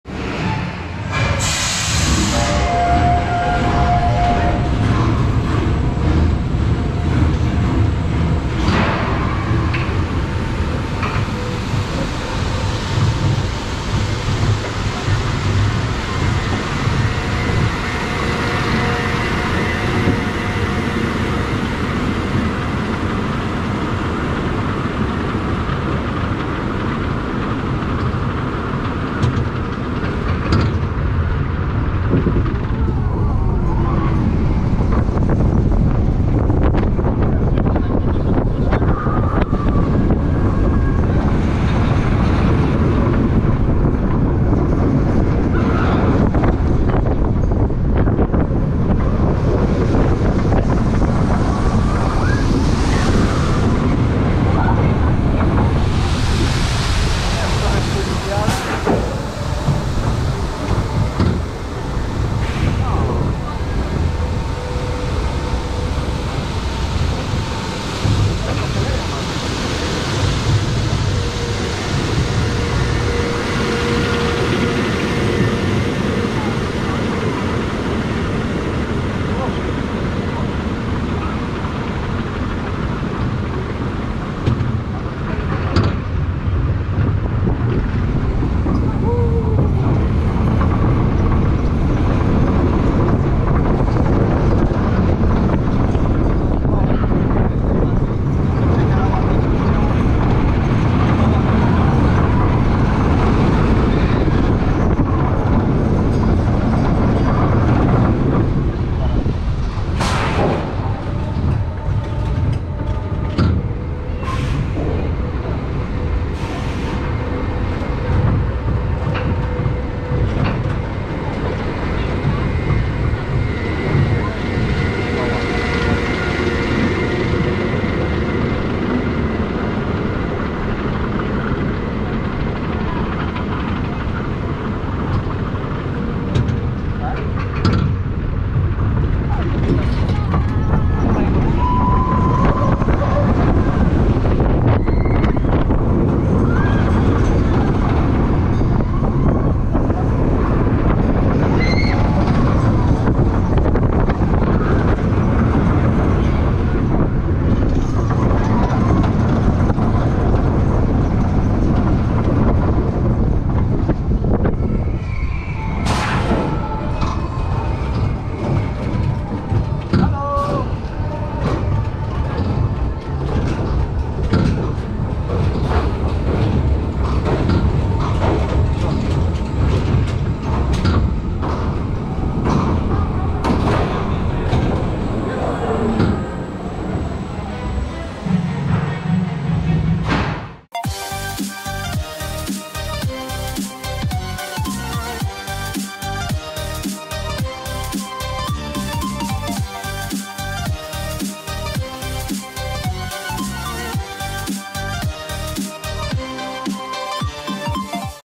Choco Chip Creek NEW Roller Coaster | Sweet Valley - Energylandia | OnRide POV
Choco Chip Creek is a new roller coaster at Energylandia in Poland. The ride is located in the recently opened Sweet Valley themed area of the amusement park. Choco Chip Creek has a total of three lifthills. The ride takes roughly 3:30 minutes, due to a track length of 3,760 ft [1,146 m]. A height of 55 ft [16.5 m] and a top speed of 34 mph [55 kph] make the ride a great experience for the whole family.

Stats :
Park : Energylandia [Poland]
Manufacturer : Vekoma
Type : Mine Train [Roller Coaster]
Height : 55 ft [16.5 m]
Length : 3,760 ft [1,146 m]
Inversions : none
Max. Speed : 34 mph [55 kph]